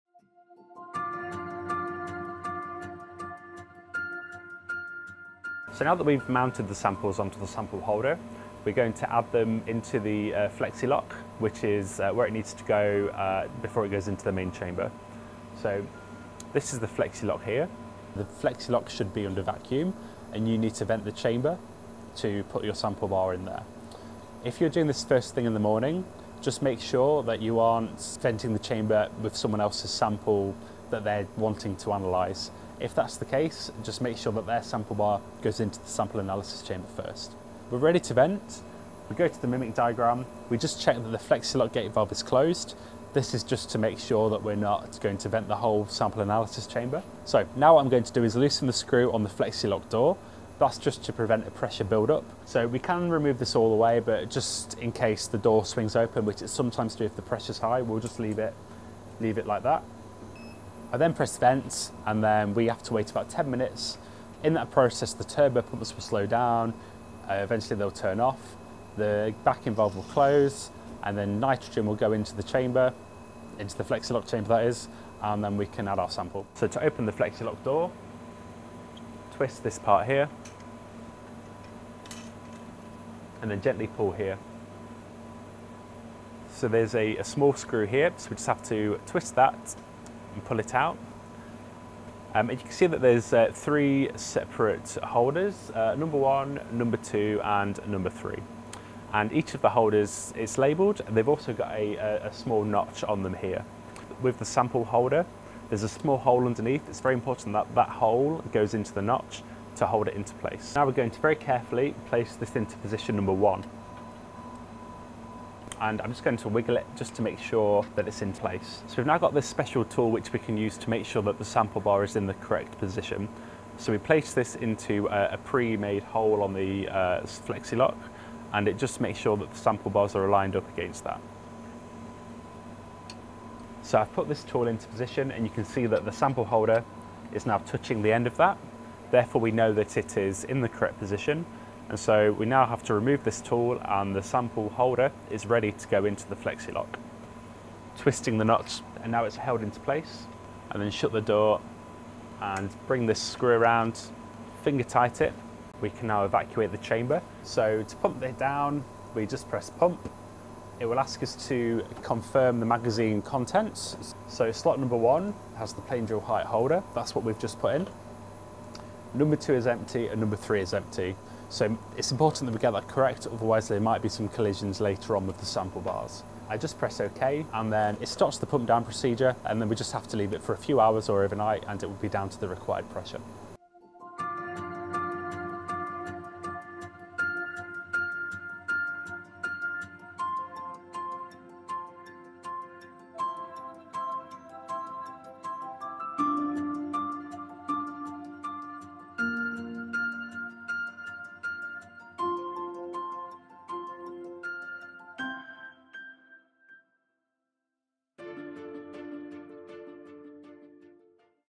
XPS course: 4. Sample Mounting into Flexi Lock Kratos Axis Supra Plus XPS
Part 4 of a 5 part training series on X-ray Photoelectron Spectroscopy. Presented by Dr Craig Stoppiello from the Centre for Microscopy and Microanalysis at The University of Queensland.